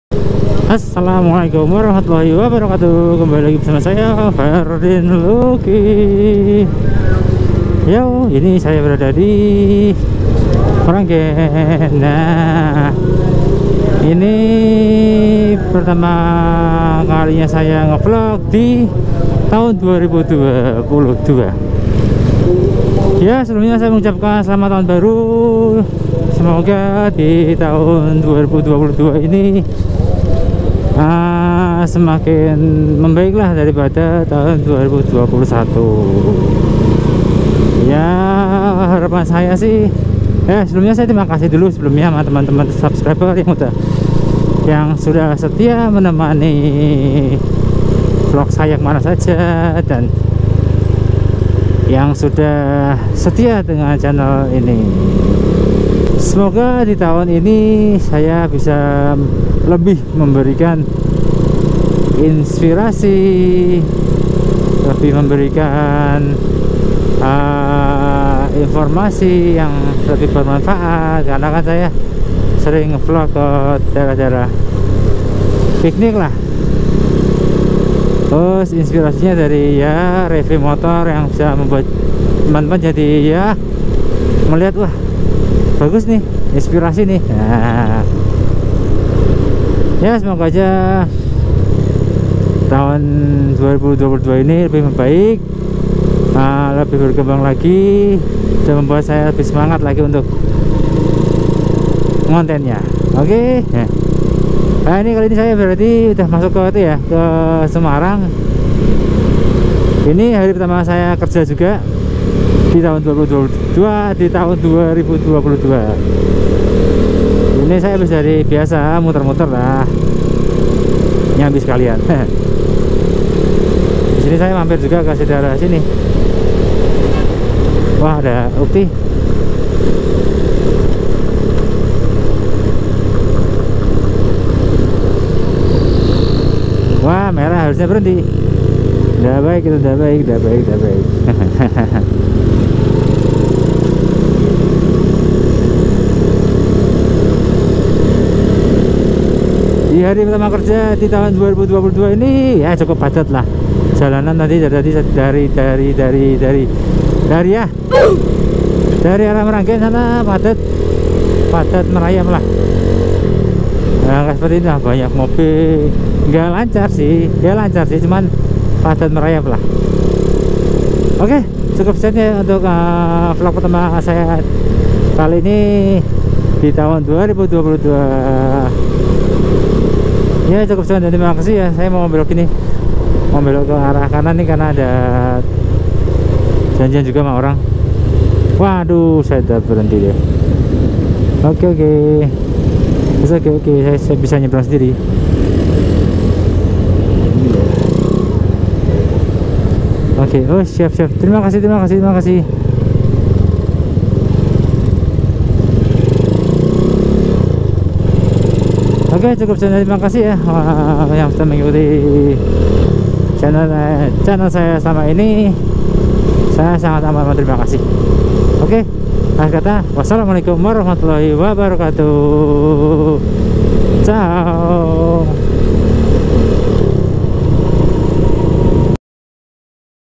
Motovlog #38 | Spirit Tahun di Tahun 2022
Motovlog kali ini hanya Ucapan terimakasih kepada teman teman yg selama ini di tahun 2021 sudah Setia pada channel "Ferdin Lukiey"